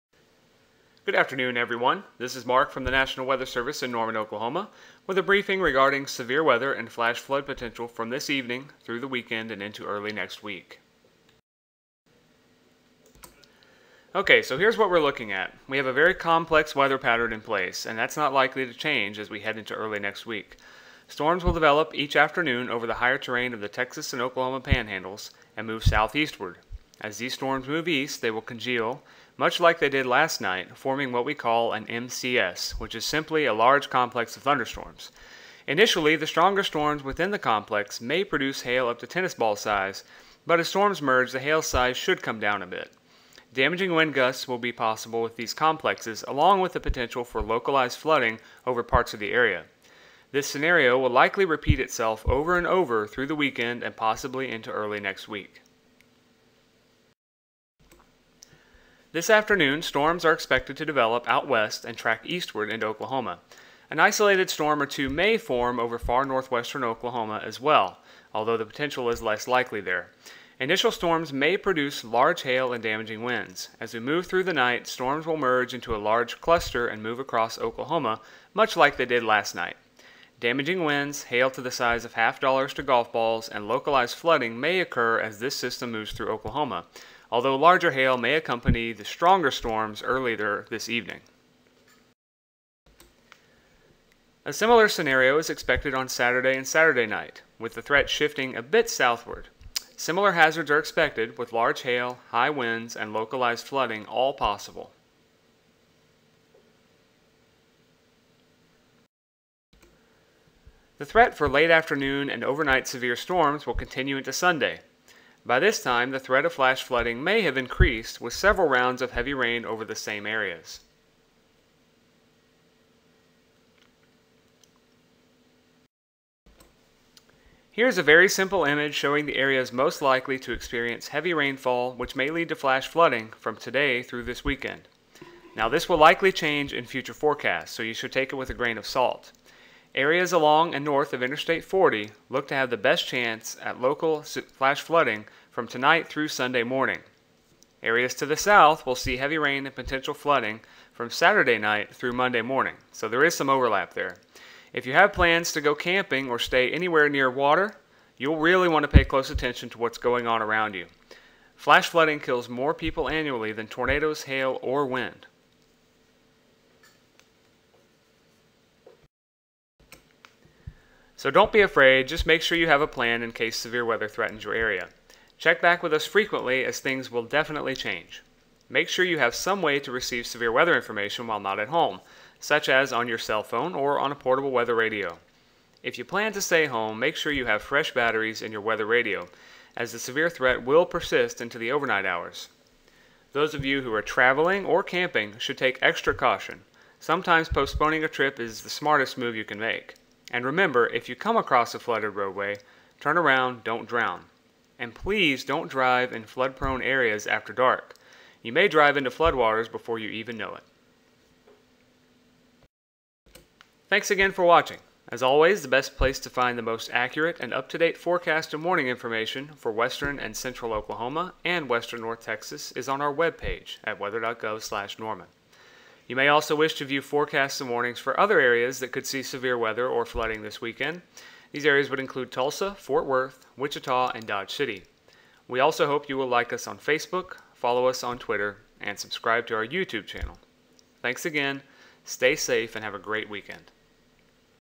Severe Weather Briefing - June 6, 2014 @ 12:00 PM CDT
A short briefing regarding severe storm and flash flood potential from Friday through the upcoming weekend and into early next week.
National Weather Service
Norman, Oklahoma
weather.gov/norman